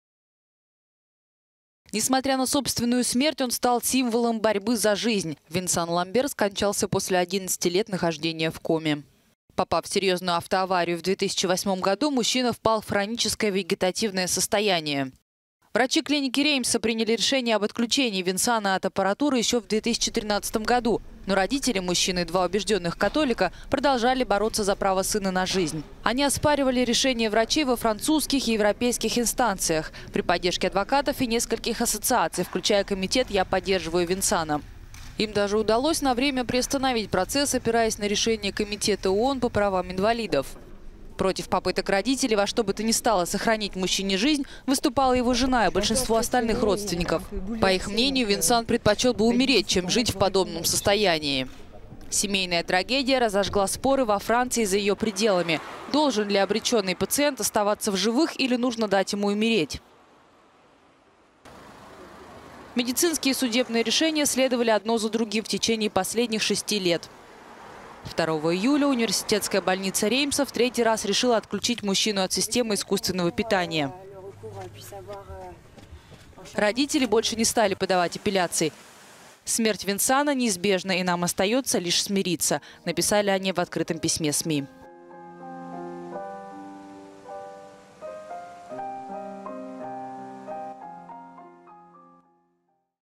Семья Венсана Ламбера 11 лет боролась за его жизнь в суде
Семейная трагедия разожгла споры во Франции и за ее пределами. Должен ли обреченный пациент остаться в живых или нужно дать ему умереть? …